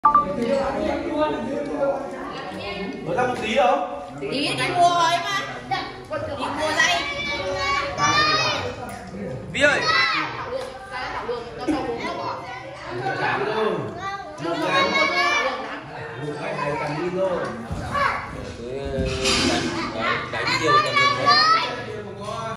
ăn tết nhà bác lâm hồng 2023 Vietnamese Cute Baby Boy Coca
My cute baby boy is Coca, he was born at October 18, 2018, he is very smart and lovely